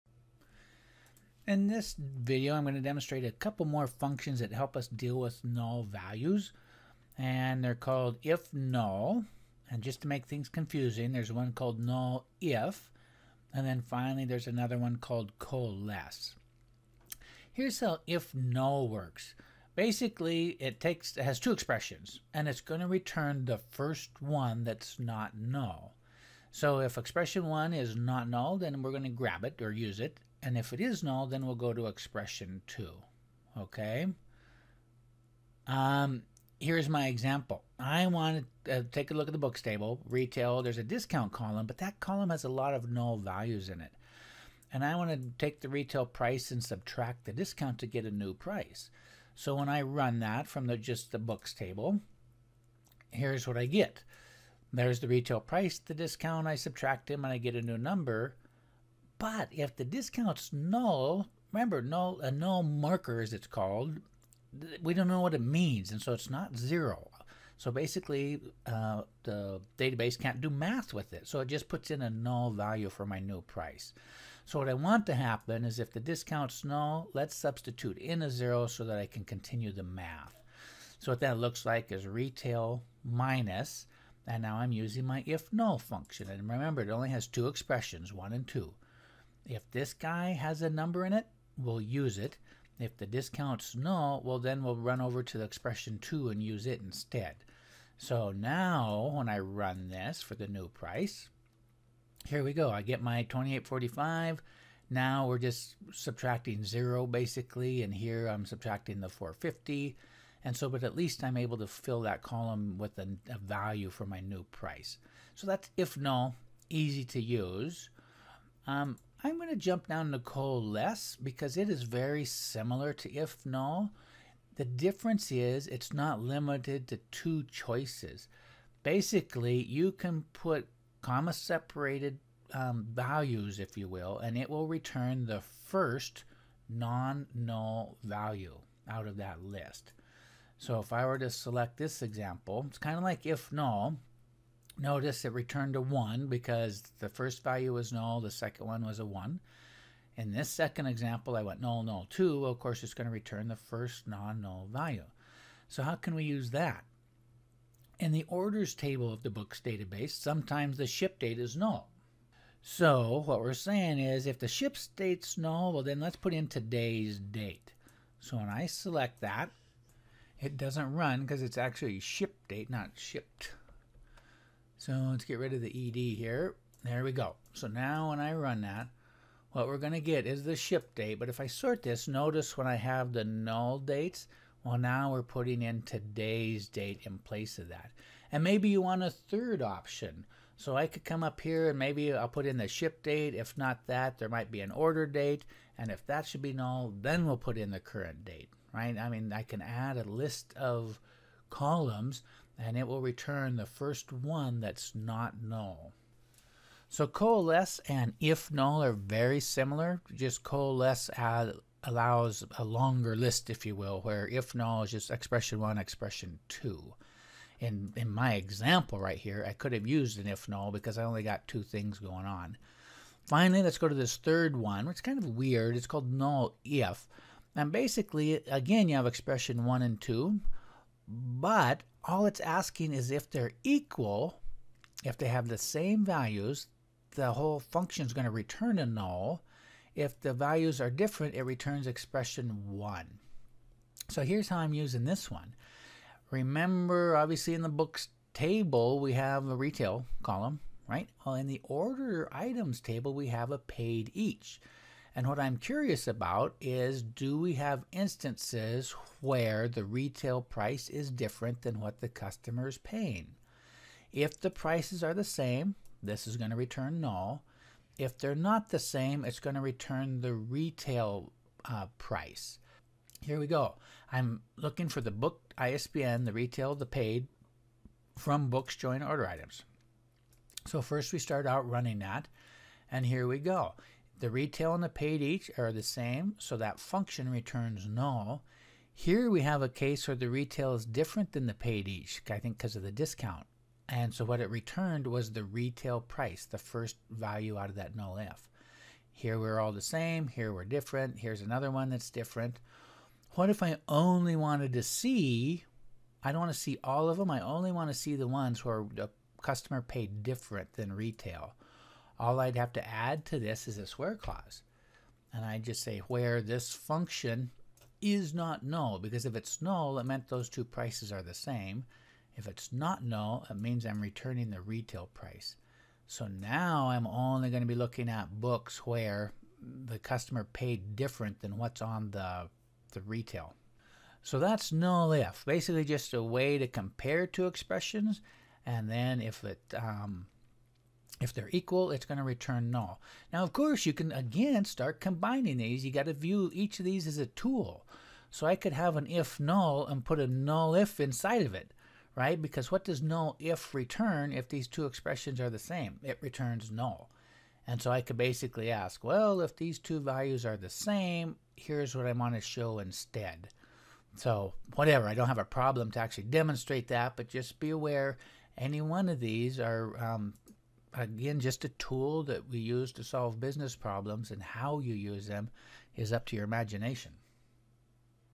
Coalesce_IfNull
Demonstrate Coalesce, IfNull, NullIf in mysql